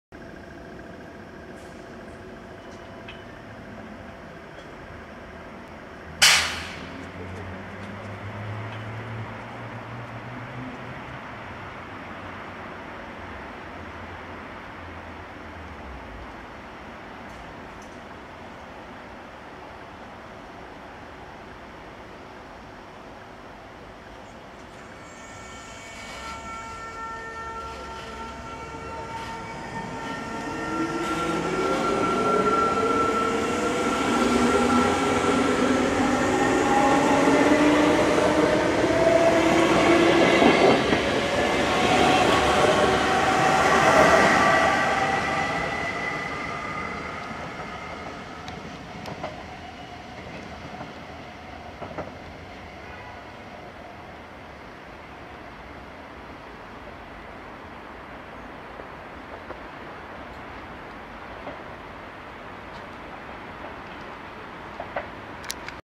NS SGMm 2115+2945 vertrekken van station Tilburg
NS SGMm 2115+2945 vertrekken van station Tilburg als Sprinter 6646 naar 's-Hertogenbosch

15 januari 2017

Reageer en/of geef een duimpje omhoog als je wil!